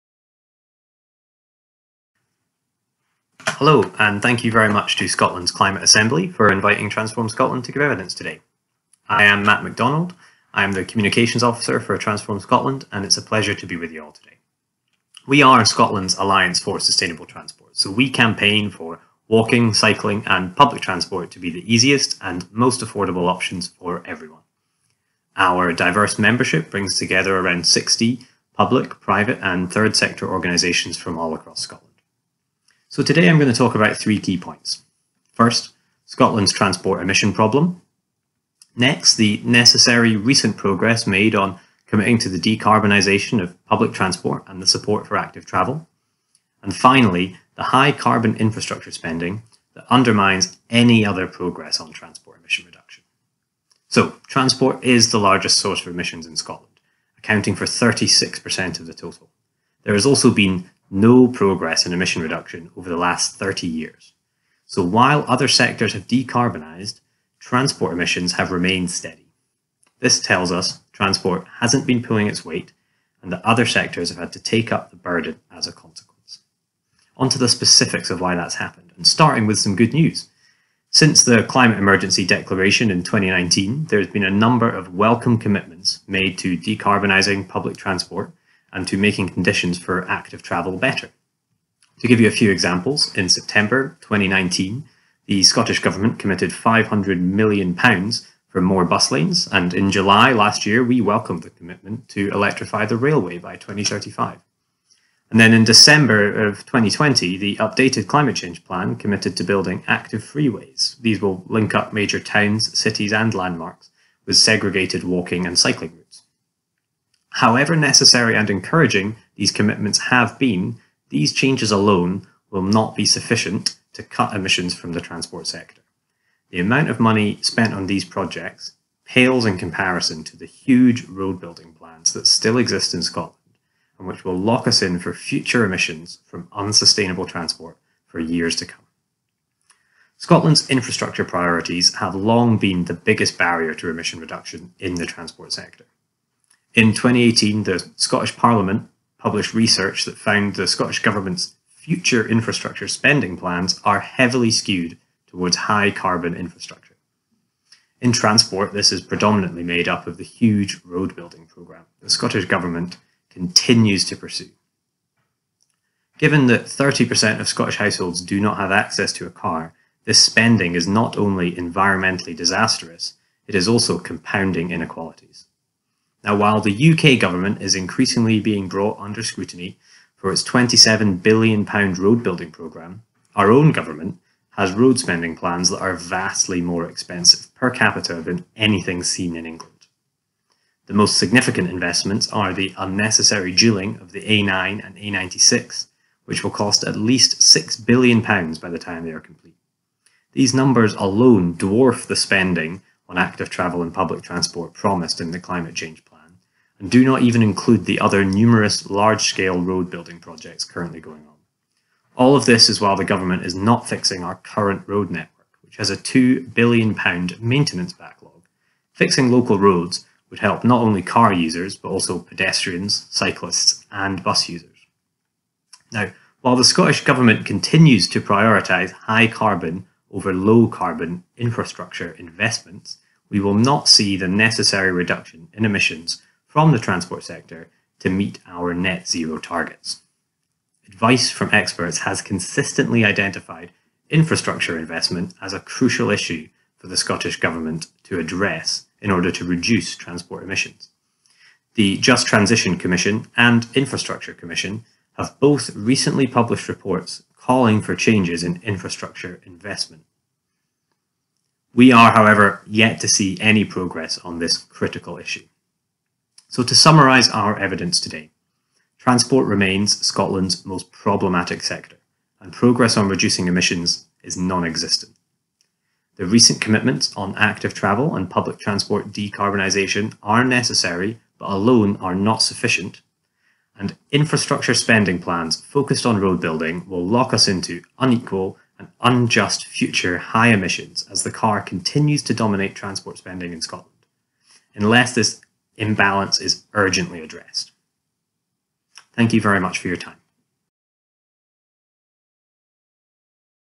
Land Travel with Matt McDonald | Scotland's Climate Assembly
On Saturday 9th January, Matt McDonald, from Transform Scotland, looked at how Scotland can change how it travels over land to help tackle the climate emergency.

For weekend 3, the Assembly broke into three groups to enable in-depth discussion of specific topics. Groups considered either: diet and land use, new and existing homes or surface and air travel.

Scotland’s Climate Assembly brought together people from all walks of life, from across Scotland, to learn about and discuss how we can tackle climate change.